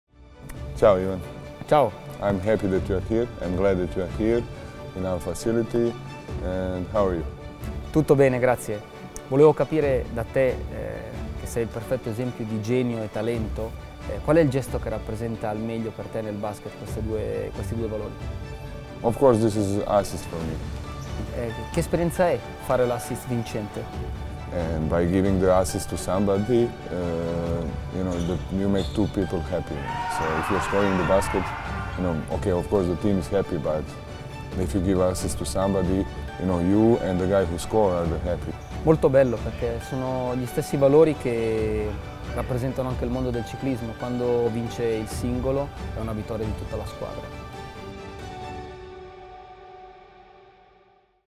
UN ASSIST AL TALENTO.
Con MilosTeodosic non si può che pensare al TALENTO. Il nostro Ivan Basso ha parlato con il playmaker della Virtus Segafredo Bologna di assist e felicità.🔥🏀

👉 vivi le esperienze dei campioni, segui